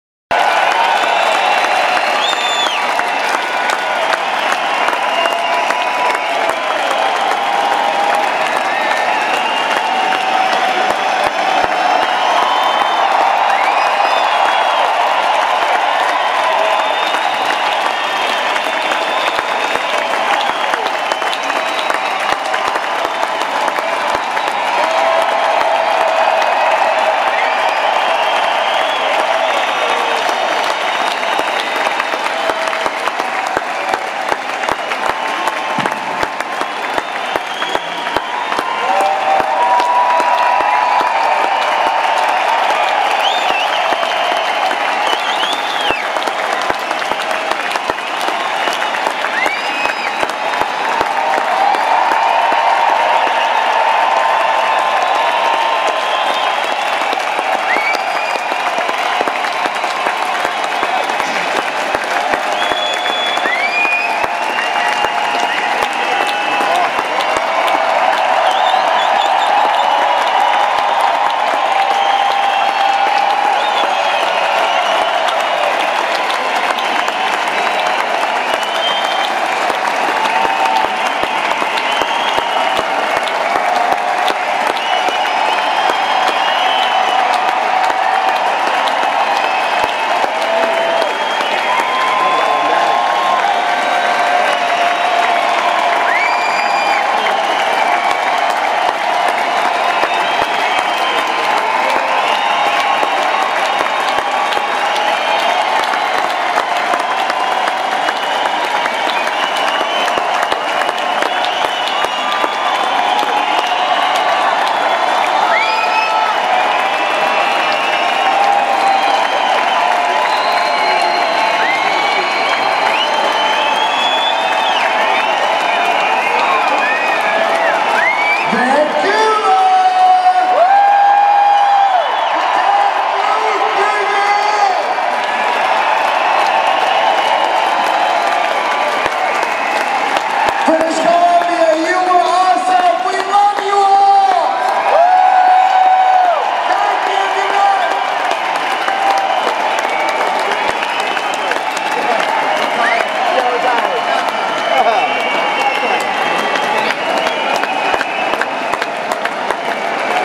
Chicago / Earth, Wind & Fire - Bows
Performed at Rogers Arena, Vancouver on November 7, 2016